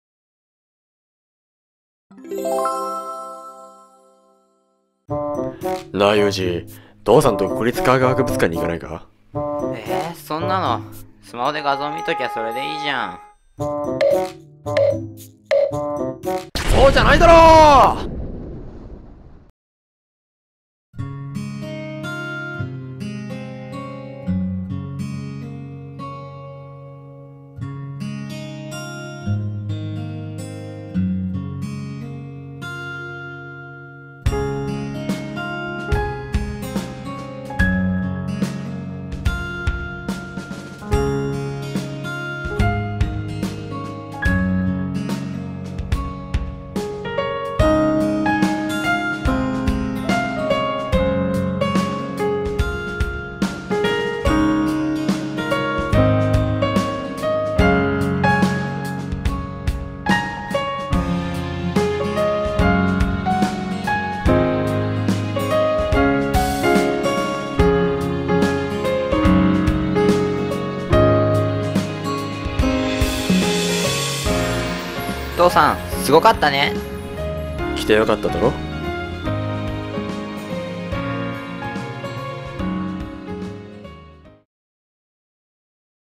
高校生作成のかはくＰＲ動画を公開！（制作：山村学園高等学校オーディオビジュアル部）
国立科学博物館が6月1日から再開しました！
このたび、埼玉県の山村学園高等学校オーディオビジュアル部の皆さんに国立科学博物館の魅力を伝えるＰＲ動画を作成していただきました。
部員の皆さんが実際に館内で撮影し、丹念に編集して作った力作をぜひご覧ください！

高校生以下の児童生徒の皆さんは、常設展示の入館料が無料です！
動画を見てかはくのことが気になった皆さん、ぜひお越しくださいね。

※国立科学博物館は令和2年6月1日(月)より再開しました。再開にあたって、上野本館は予約制とさせていただきます。
詳細はＨＰをご覧ください。